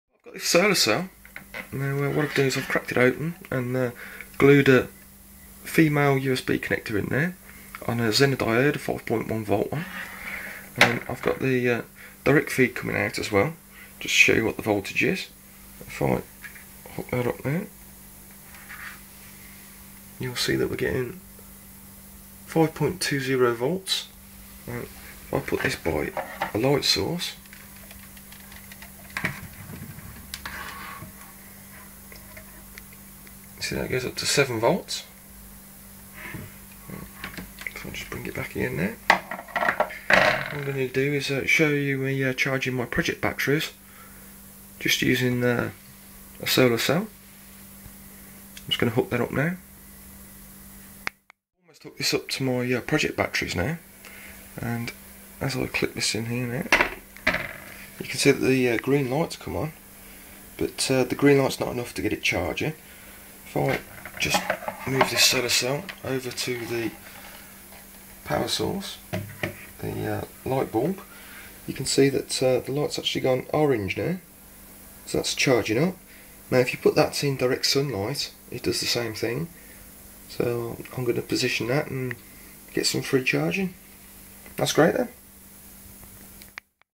Project Batteries (Solar Cell) 003
Charging my Project Batteries using a Solar Cell